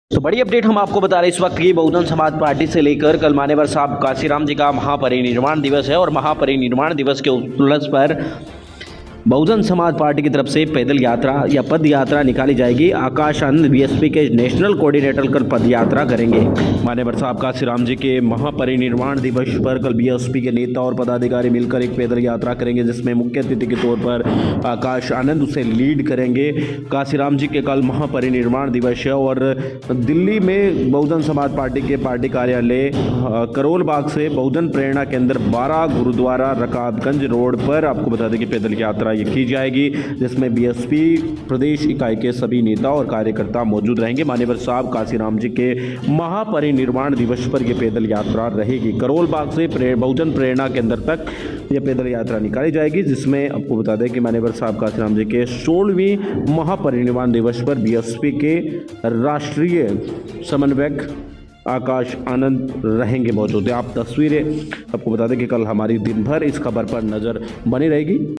मान्यवर साहब कांशीराम जी के महापरिनिर्वाण दिवस पर कल आकाश आनंद करेंगे पदयात्रा || BSP
हमसे जुड़े -